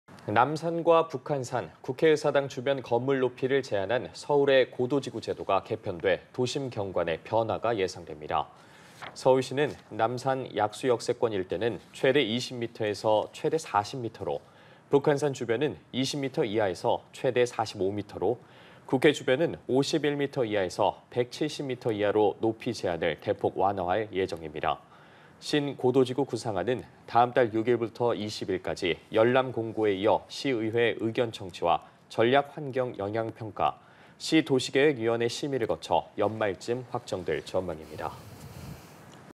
서울 고도지구 높이제한 완화…북한산 45ｍ·여의도 170ｍ / 연합뉴스TV (YonhapnewsTV)
서울 고도지구 높이제한 완화…북한산 45ｍ·여의도 170ｍ

남산과 북한산, 국회의사당 주변 건물 높이를 제한한 서울의 '고도지구 제도'가 개편돼 도심 경관에 변화가 예상됩니다.

서울시는 남산 약수역세권 일대는 최대 20ｍ에서 최대 40ｍ로, 북한산 주변은 20ｍ 이하에서 최대 45ｍ로, 국회 주변은 51ｍ 이하에서 170ｍ 이하로 높이 제한을 대폭 완화할 예정입니다.

신 고도지구 구상안은 다음 달 6일부터 20일까지 열람공고에 이어 시의회 의견 청취와 전략환경영향평가, 시 도시계획위원회 심의를 거쳐 연말쯤 확정될 전망입니다.